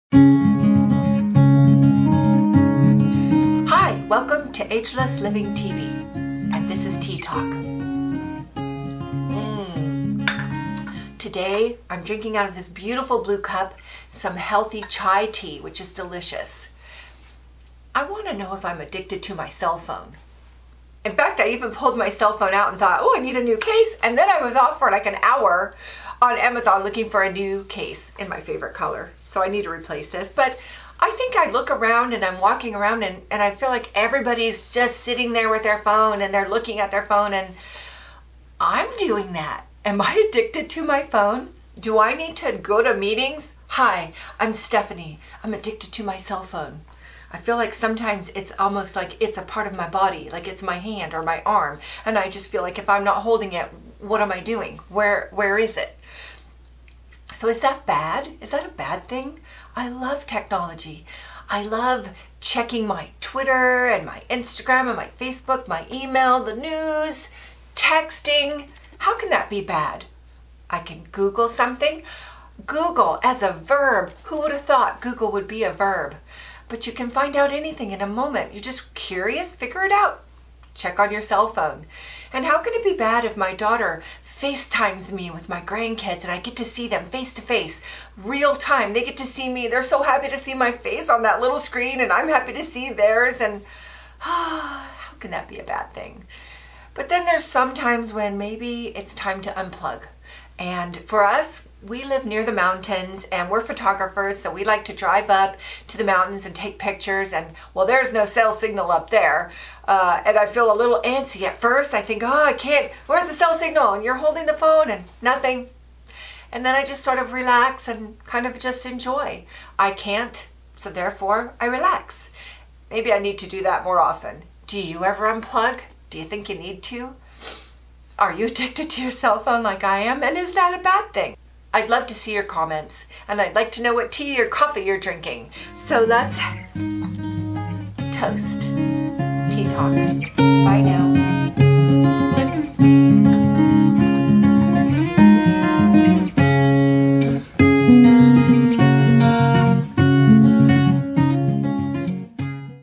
Ageless Living tv Tea Talk am I addicted to my cell phone?
Tea Talk is a short video just to say hi and chat over coffee or tea.

Please like/thumbs up and comment as I would love to hear what you think.

Thank you so much for stopping by. I truly appreciate it!

Stephanie